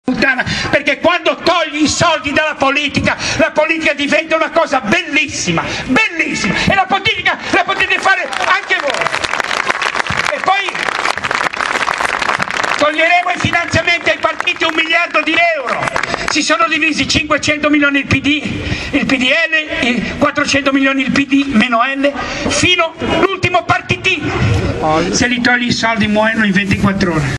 Grillo per una politica senza soldi!! togliamo i soldi dalla politica!!
Riprese effettuate dall'Associazione CRETA Pisa, a Fucecchio il giorno 28/11/11

incontro con Paul Connet, Beppe Girllo e Rossano Ercolini, non agli inceneritori e ai pirogassificatori!! l'energia prodotta è inferiore all'energia che si ricaverebbe dal solo riciclaggio (dati di Paul Connet)..bruciare i rifiuti è uno spreco oltre che un danno per la salute dei cittadini che respireranno poi le polveri sottili prodotte!!